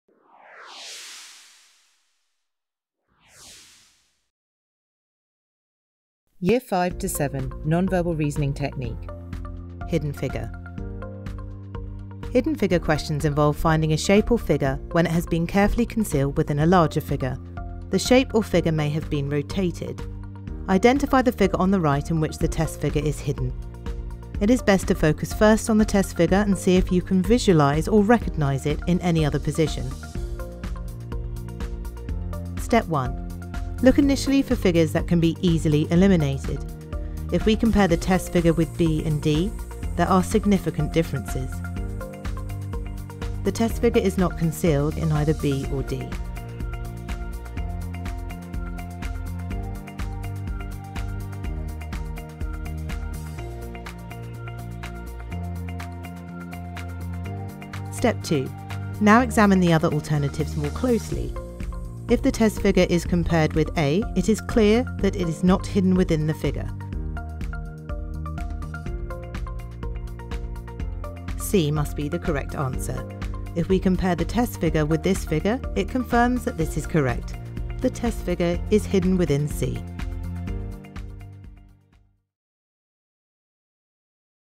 Non-verbal Reasoning: Hidden Figure (Year 5-7 - Workbook 6)
This workbook teaches non-verbal reasoning technique for recognising shapes, rotation, reflection, analogies, similarities and matrices. Each technique has a clear explanation, with a worked example, followed by plenty of practice questions. The workbook also teaches multiple-choice technique for answering non-verbal reasoning questions.

To purchase this workbook follow this link below and also view our full range.